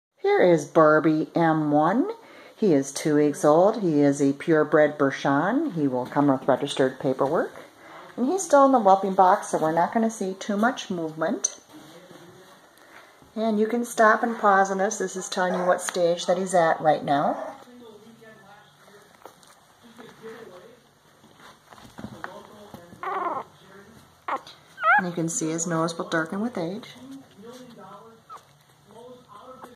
barbie09122018m1 on 9 29 2018 bichon
Bichons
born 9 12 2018
barbie m1 is available $575
0 is SOLD $575
0 are available $725
0 are SOLD $775
m=male f=female
mother 10.5# father 8#
range 8-12#
7 weeks on 11/3
9 weeks pickup 11/17
non-shed hypo-allergic
2   weeks